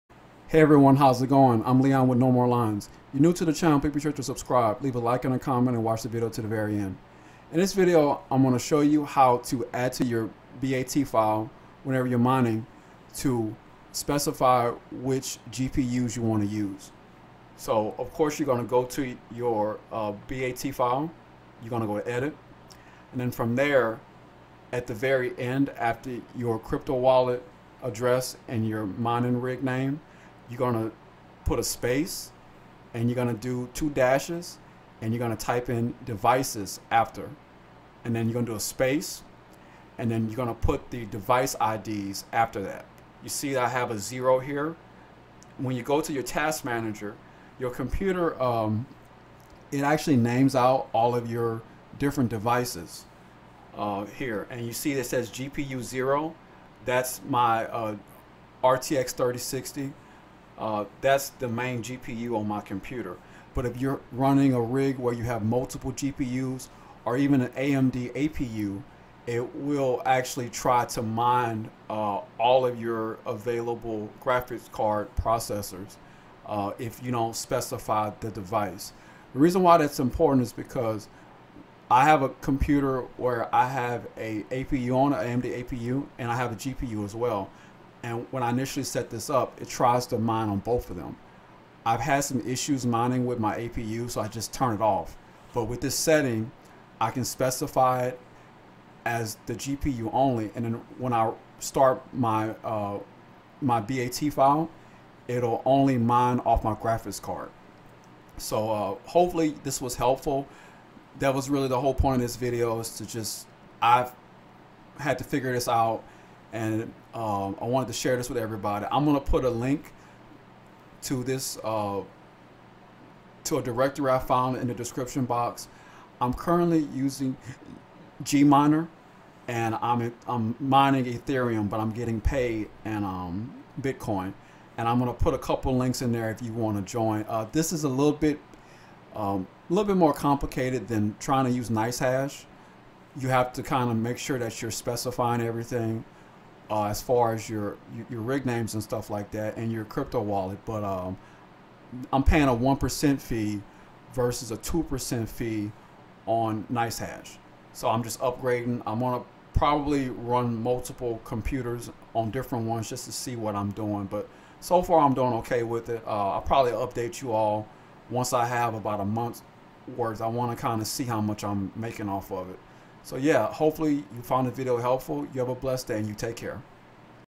GMiner | How to Specify Which Devices to Mine With (Windows)
Add the code " --devices " and the device number on the same line as your crypto wallet address and rig ID.

'--devices 0" would mean the default GPU plugged into your first PCI-E slot on your motherboard.

'-- devices 0 1" would mean GPU 0 and GPU 1 (the first and second GPUs plugged in).

Use the Task Manager will give you a list of the recognized GPUs on your rig.

Here is a resource of commands to add to your .bat file.